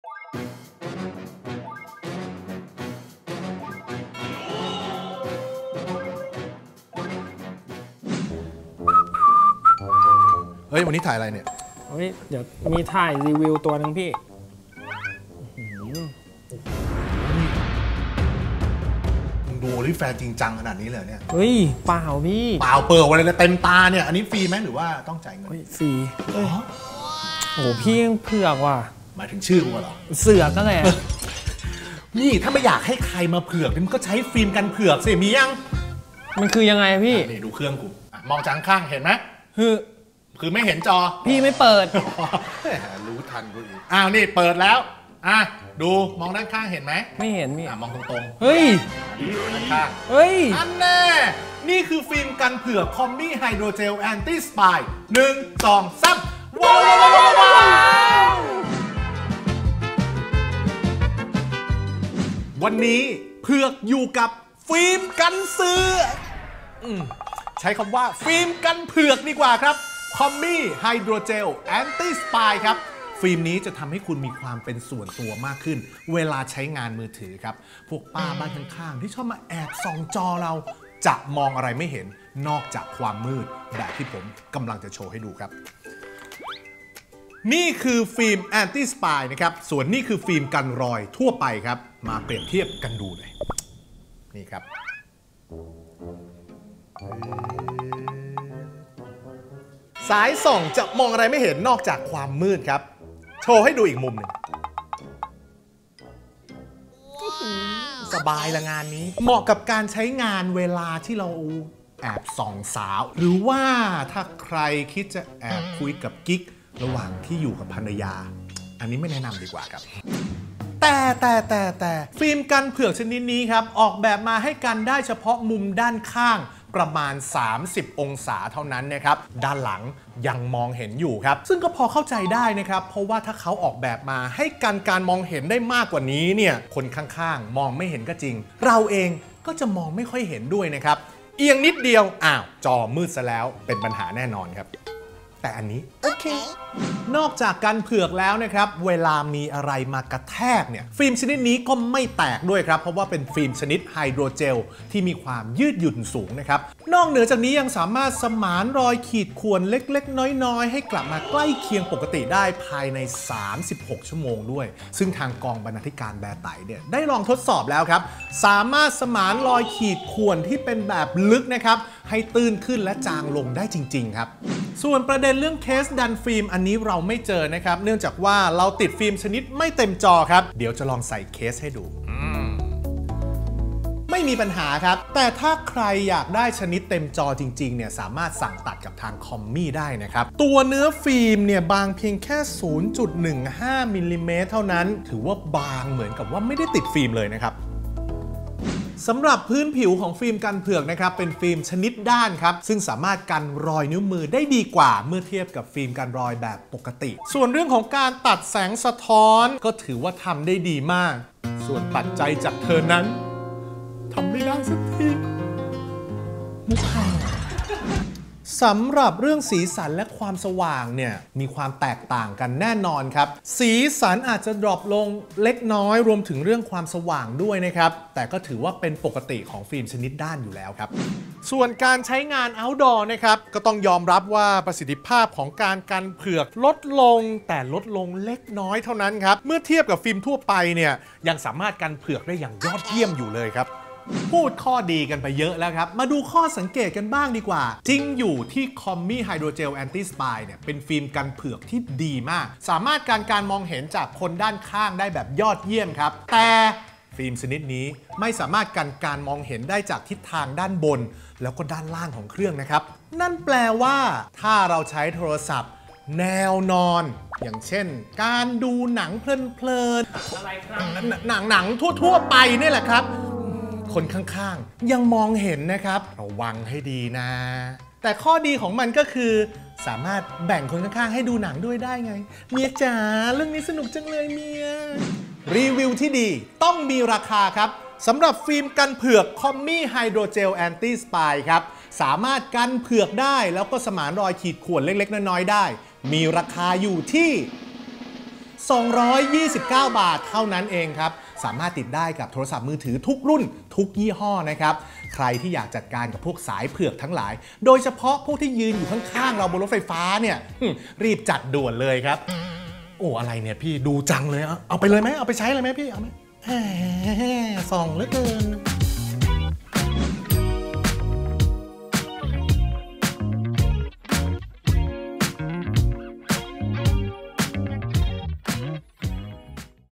รีวิวฟิล์มกันเผือก Commy Hydrogel Anti Spy
เบื่อไหมกับพวกชอบส่องมือถือคนอื่น เผลอเป็นต้องมองจอตลอด วันนี้ เผือก-พงศธร จะมารีวิวไอเทมกันกำจัดคนพวกนี้ นี่คือ ฟิล์มกันเผือก Commy Hydrogel Anti-Spy จะน่าสนใจยังไง กันเผือกได้แค่ไหน ไปดูกันในคลิปนี้

ราคาสุดปัง! ฟิล์มไฮโดรเจล Anti-Spy จาก Commy ราคาชิ้นแรก 229.-  ซื้อชิ้นที่ 2 เพียง 99.- เท่านั้น
โปรนี้ ถึงวันที่ 15 ส.ค.65 เท่านั้นนนน
กันเผือกได้แล้ววันนี้ ช้อปเลย!

LINE:           @beartai